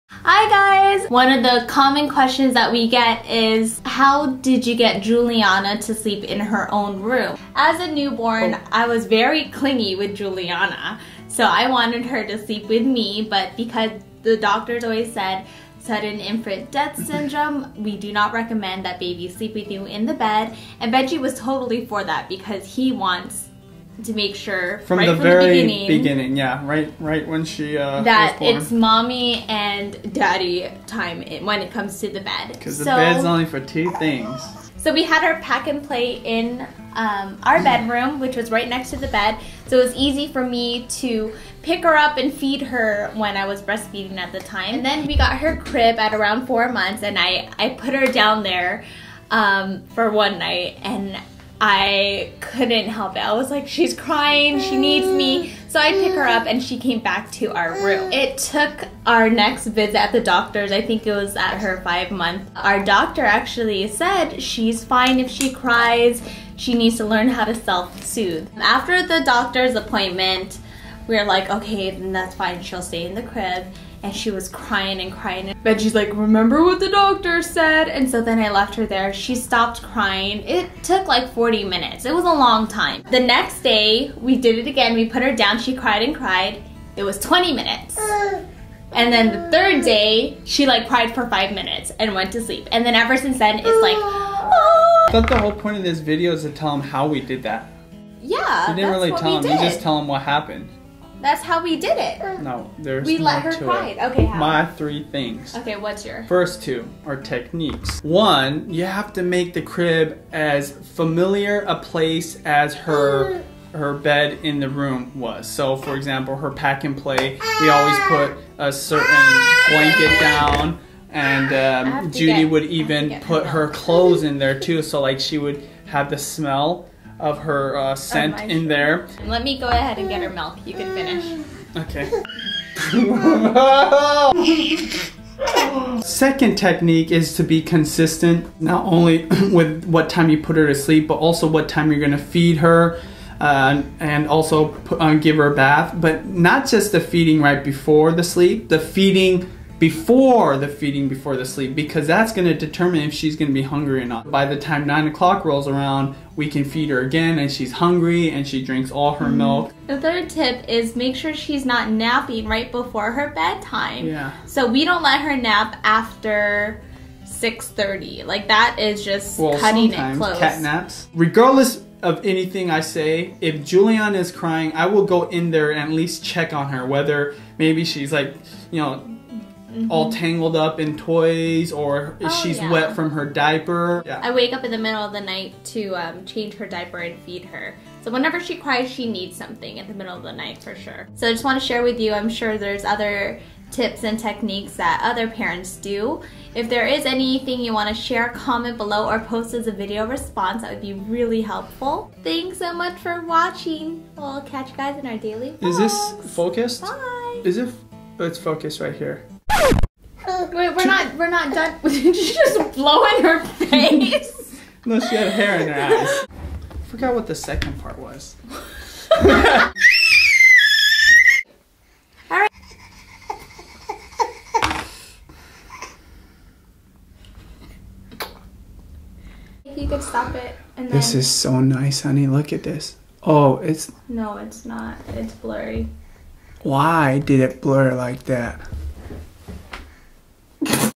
How to make baby sleep in crib - itsjudyslife
This is how we got baby Julianna to sleep in her own room! Please share tips on how you got your baby to sleep in their crib in the comments below or as a video response!